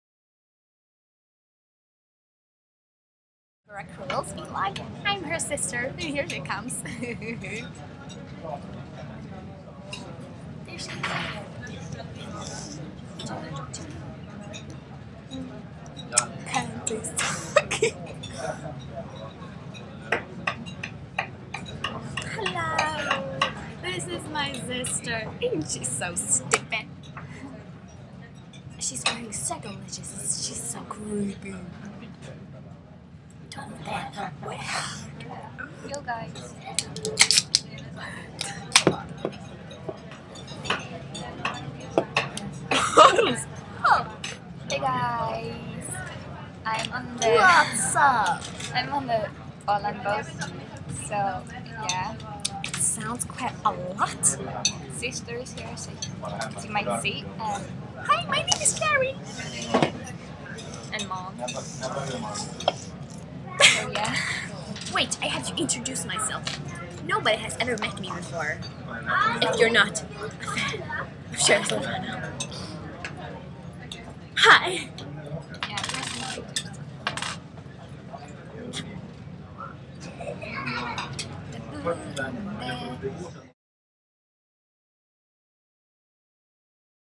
☆THURSDAY☆ - On the boat.
It was really boring haha... so i decided to record this video to kill the
time 8D
Ella, my sister is introducing herself haha, please visit her YT channel
"Cherrysylvana" she animates. XD
I'm wearing my Max Pure Green lenses :)